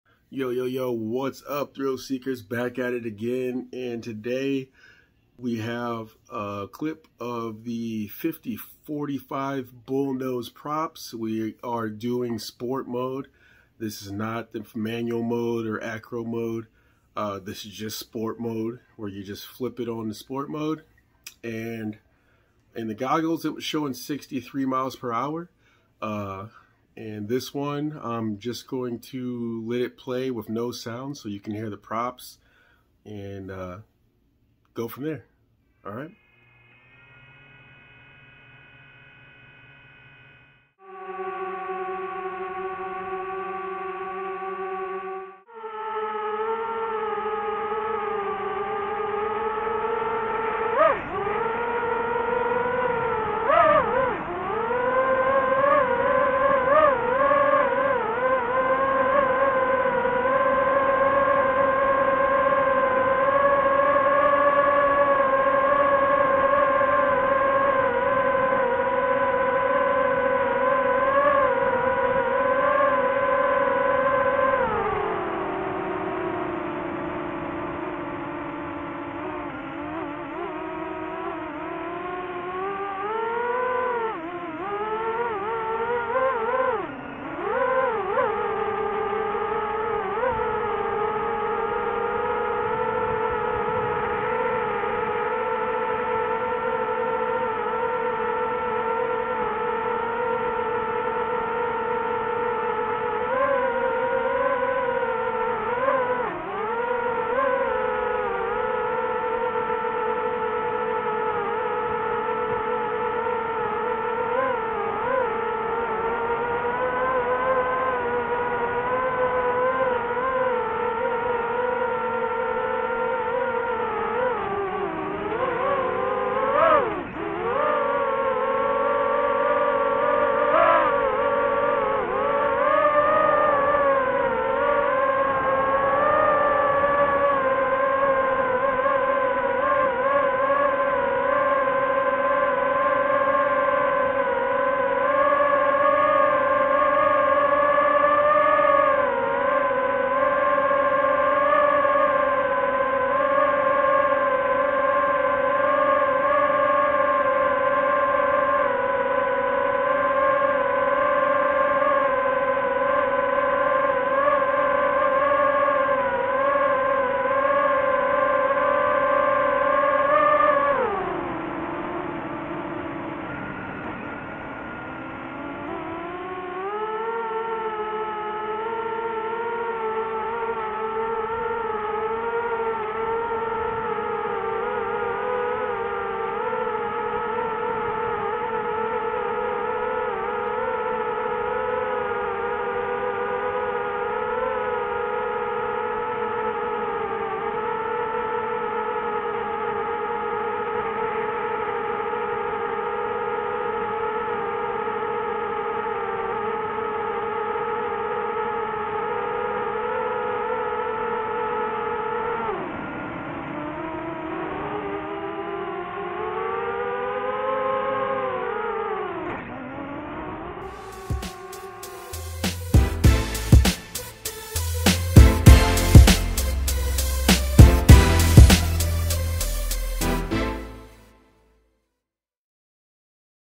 DJI FPV Drone: 5045 Bullnose Bi blades on Sport Mode 🍿
In this video we take the DJI FPV Drone for a spin in sport mode to stretch its legs 🦵